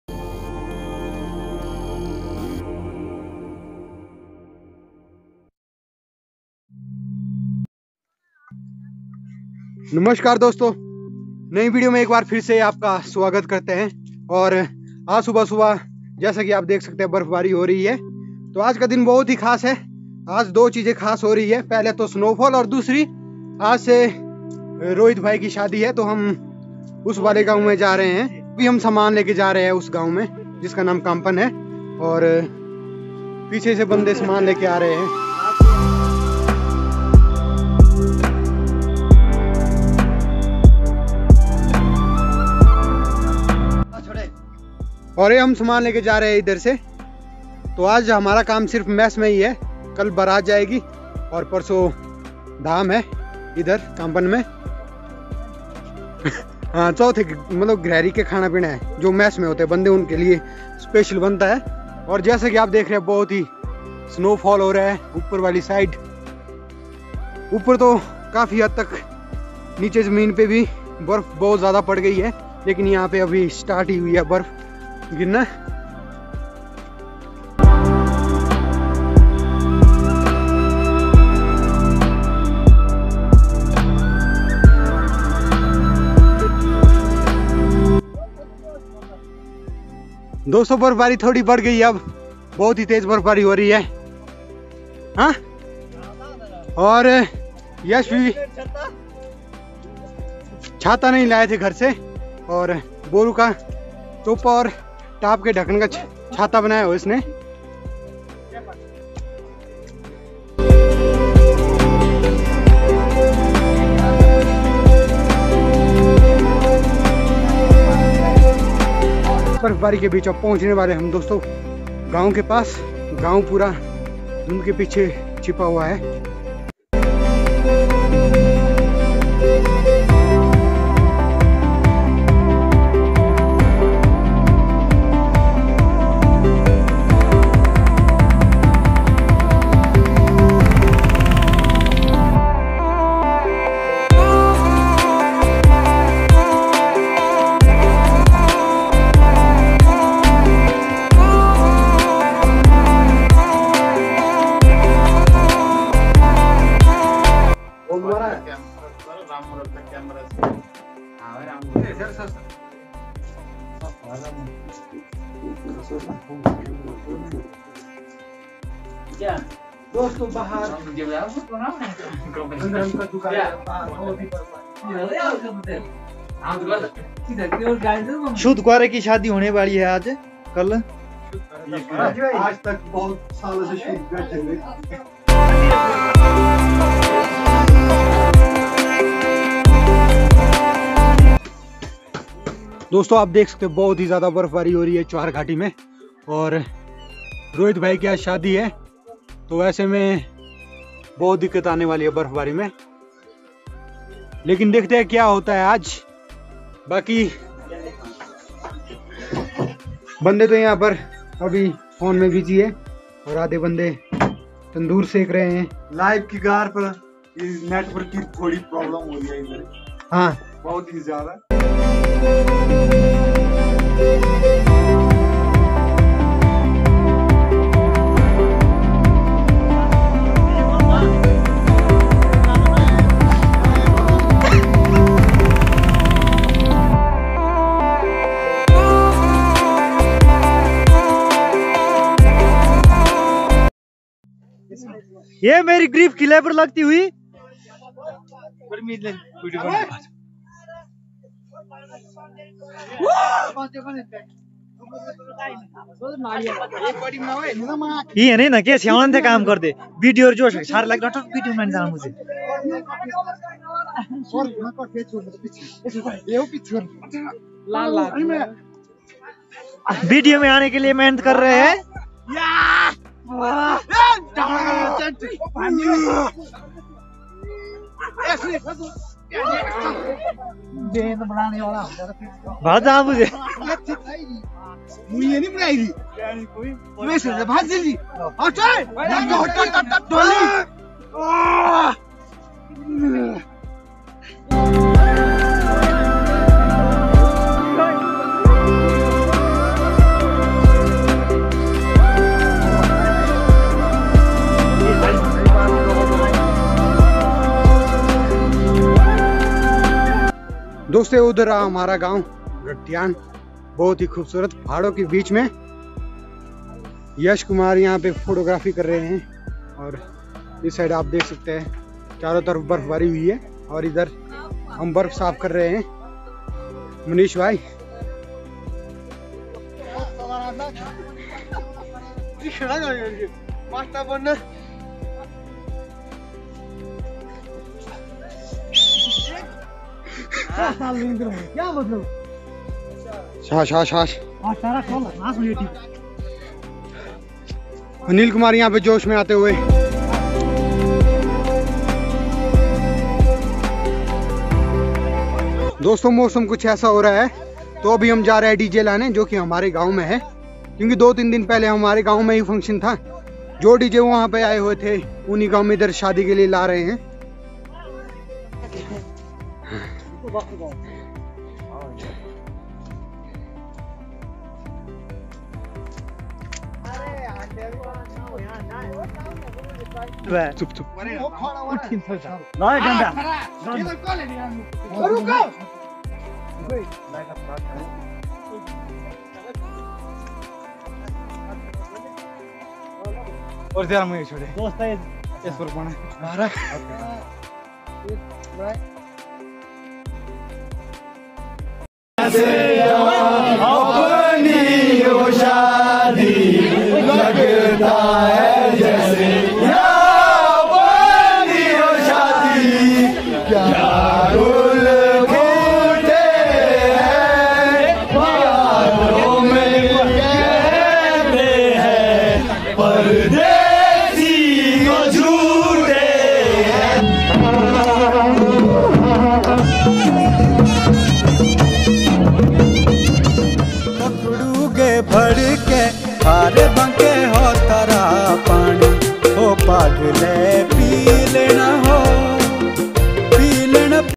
शादी के एक दिन पहले बर्फ पड़ गया 🥶 ।। choharke boyz#marriage
अक्सर शादियां सर्दियों के दिनो मैं ही होती है .क्या होता है अगर शादी के एक दिन पहले भारी बर्फबारी हो जाए .हमारे साथ भी कुछ ऐसा हुआ. जानने के लिए देखिए पूरी वीडियो।
(शादी की हार्दिक शुभकामनाएं भाई जी)
(rohit weds oma)
NEXT VIDEO SOON
ARTIST:ANXMUS

SNOWFALL DURING MARRIAGE
HIMACHAL PRADESH